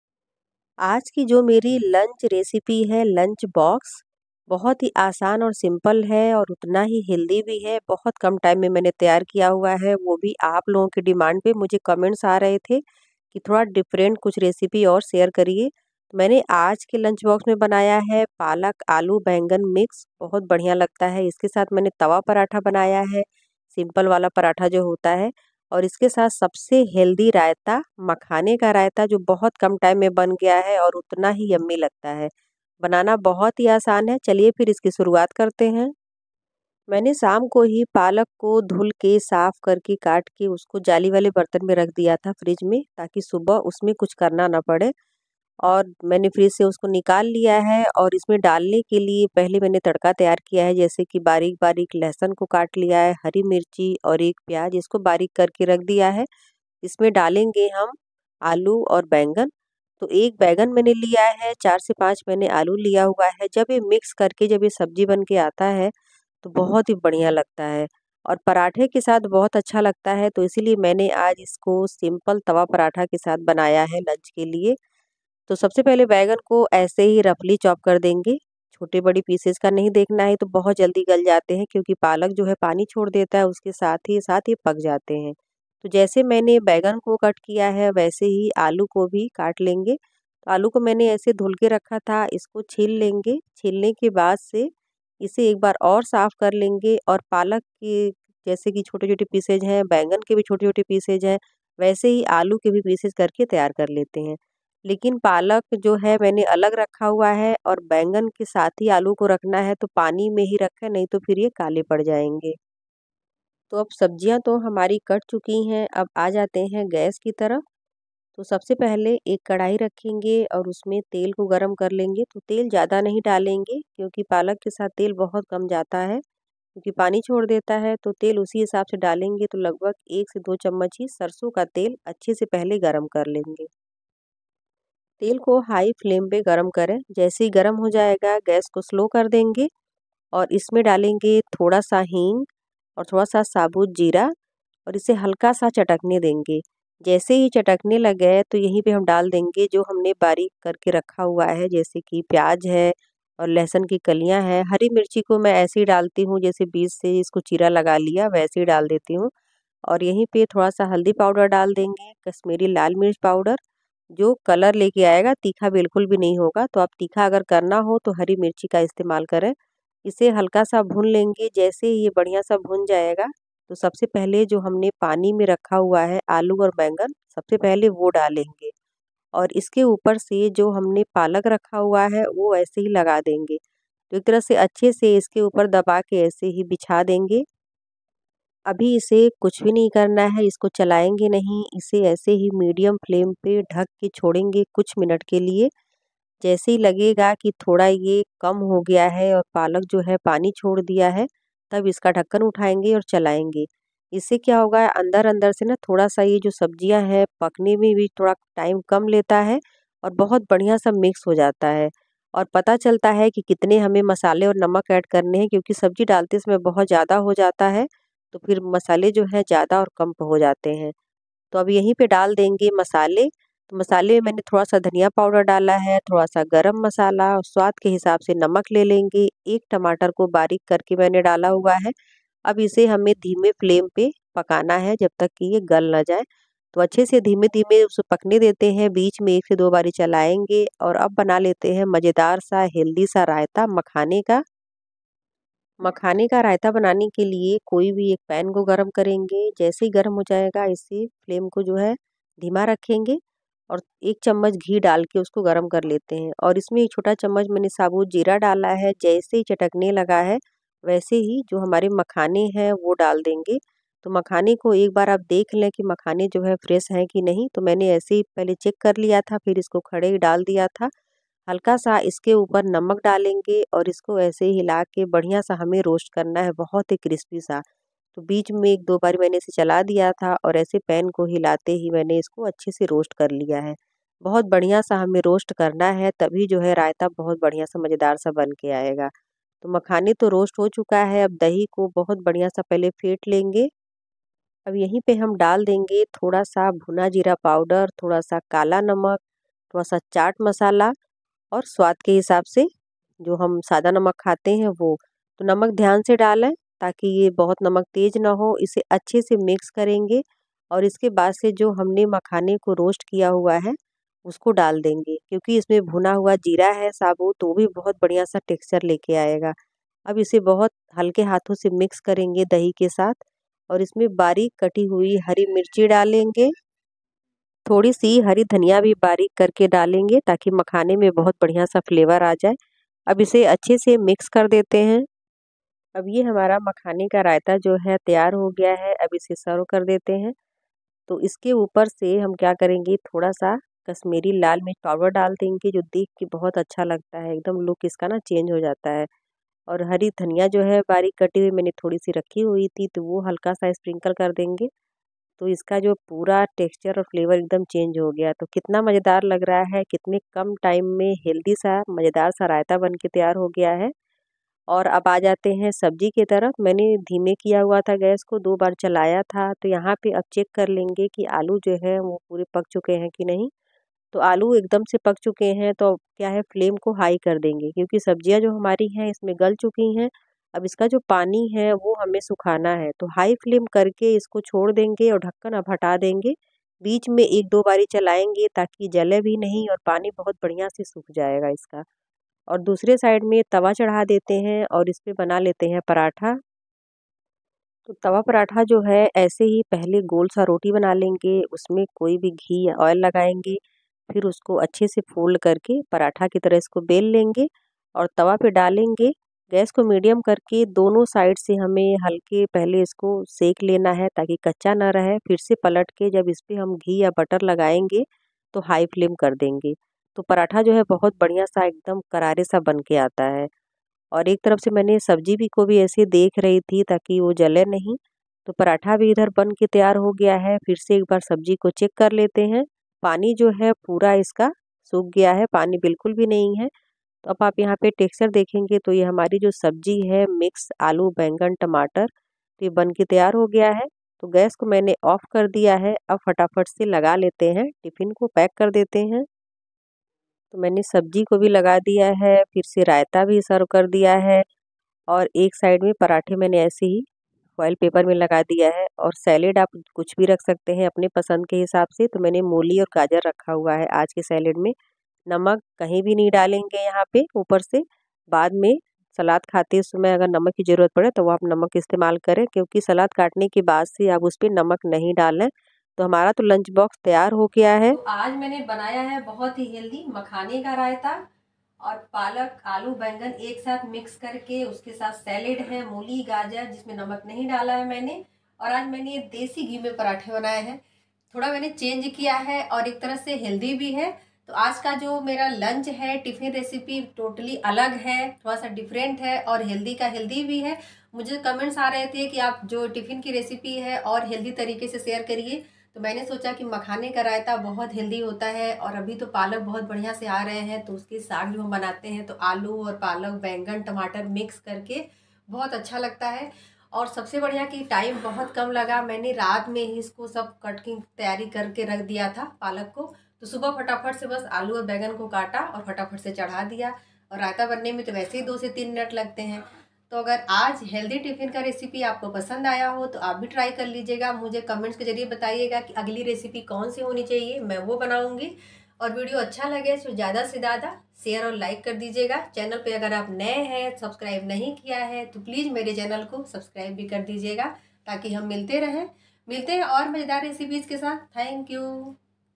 Instant Healthy Lunch Box with Special Makhana Raita | Tiffin Recipe स्पेशल मखाना रायता के साथ
Healthy Instant Lunch Recipe | Instant Healthy Breakfast Recipe |
Instant Breakfast Recipe for Tiffin | Morning Fast Tiffin | Tiffin Recipe |

सुबह की जल्दीबाजी में झटपट टिफ़िन बनाये स्पेशल मखाना रायता के साथ |
Healthy Lunch Recipe है और जल्दी बन भी जायेगा और इसका best part "Makhana Raita" है जो की Special बनता है इस Lunch Box Recipe को | तो आप भी बना ही लीजिये अब इस वीडियो को देख के |
और मेरे वीडियो को ढेर सारा प्यार दीजिये |
आप सब के प्यार और सहयोग की आवस्यकता है हमे |

धन्यवाद🙏